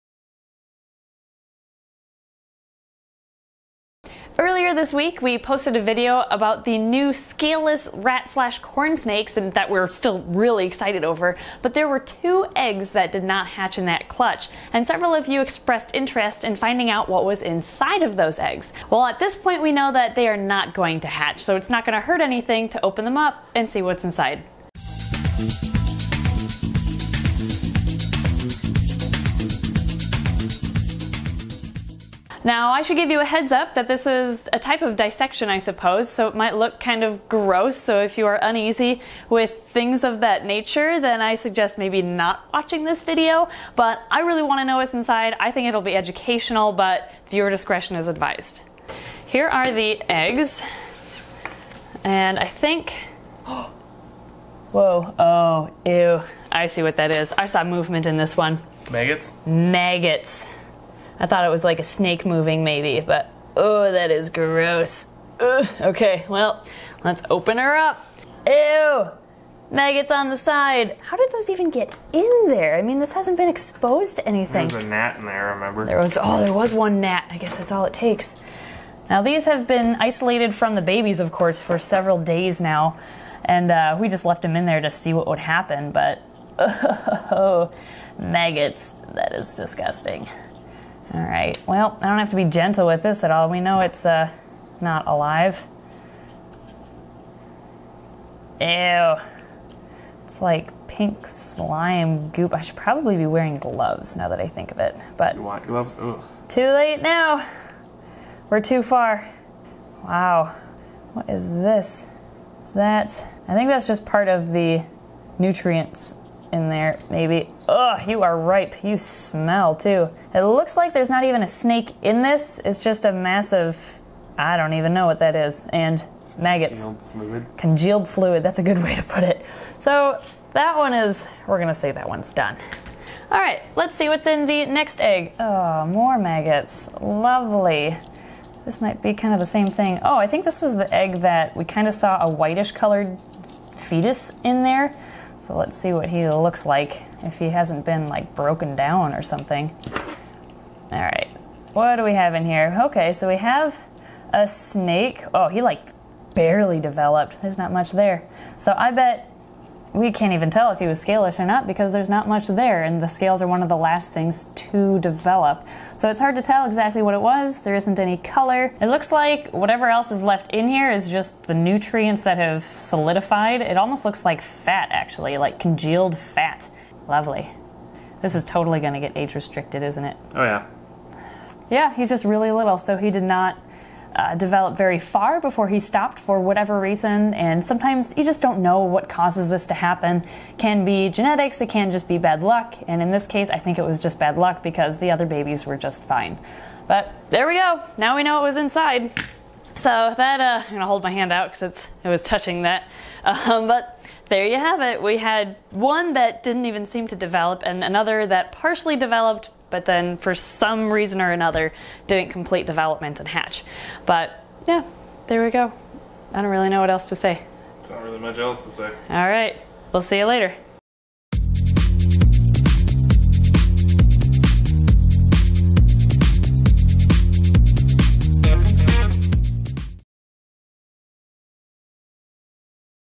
Opening up Dud Snake Eggs
I had a request to film the dissection of the 2 dud (undeveloped, dead in shell) eggs from our scaleless snake clutch. I thought it was a good idea and was curious myself about what was inside, so here you go!